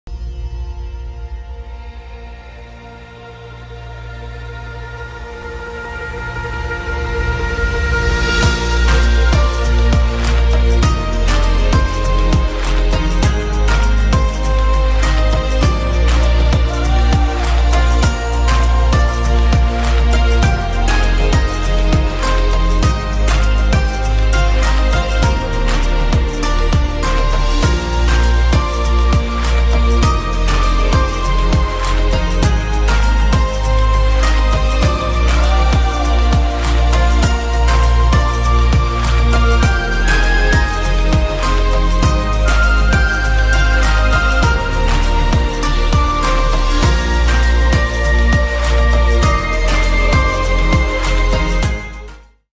Life in Ramadan | Animation
Ramadan is very important events for our muslim people. Our whole routine change in this special month. It’s a short animation video about what we do in ramadan.